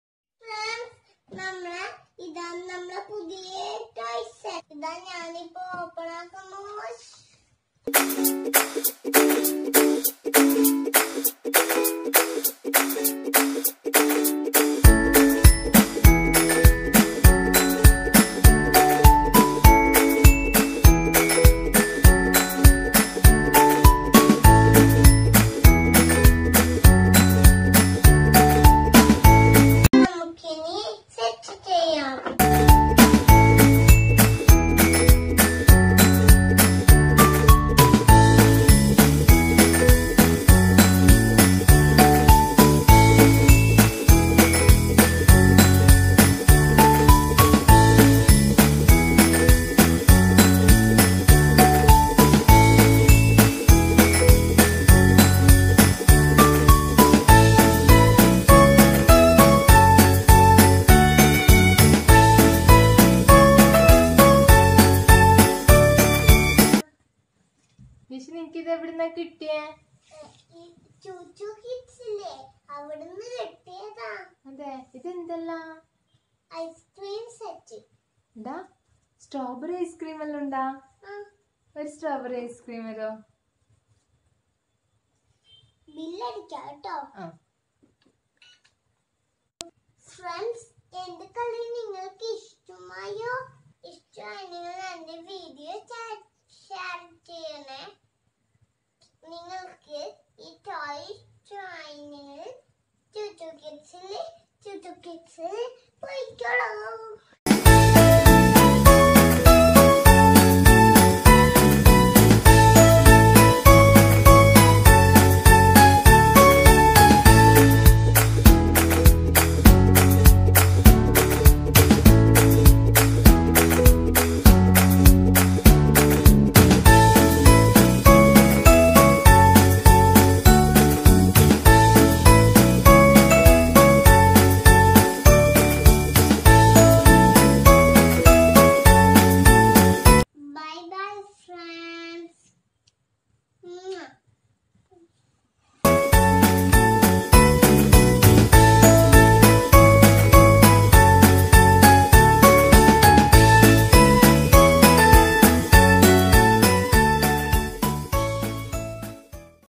Unboxing - Toy Trolly Candy Car
Unboxing Of Toy Trolly Candy Car By Jaish Muhammed Jassim